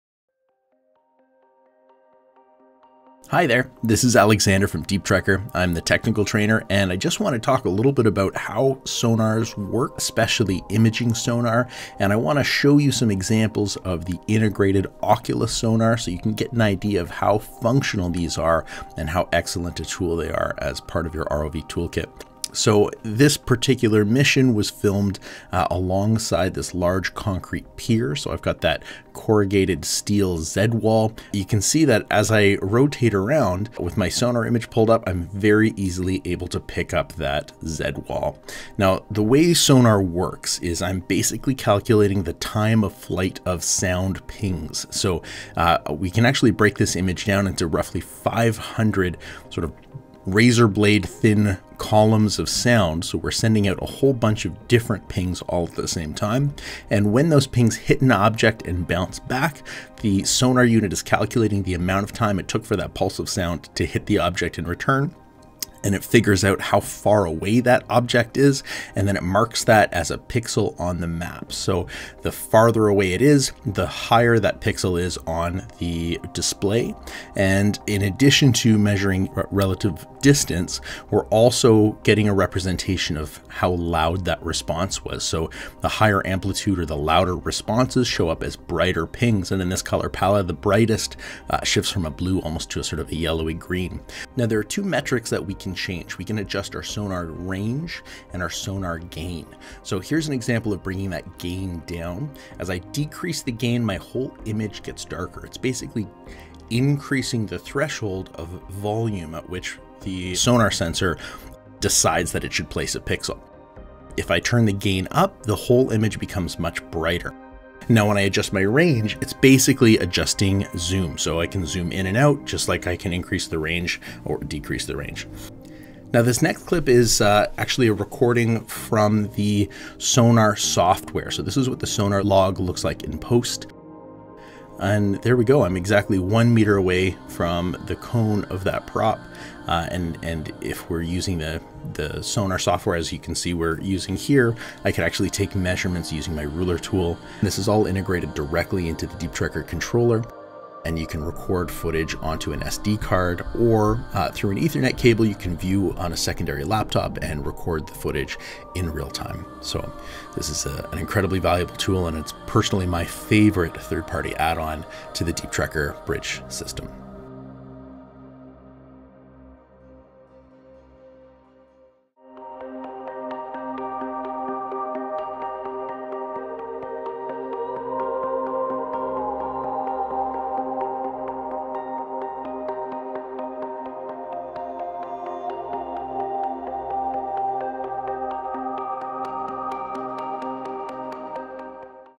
Integrated Imaging Sonar Explained | Deep Trekker
Sonar systems have many applications, but what should you know? Deep Trekker's Technical Trainer Alex explaining how imaging sonars work with underwater ROVs.  Examples with integrated Oculus Imaging Sonar on a concrete pier, and propeller cone.

Chapters:
0:00 Intro
0:38 How Sonars Work
1:34 Sonar Settings: Range & Gain
2:05 Recording Sonar Data on BRIDGE
2:47 Outro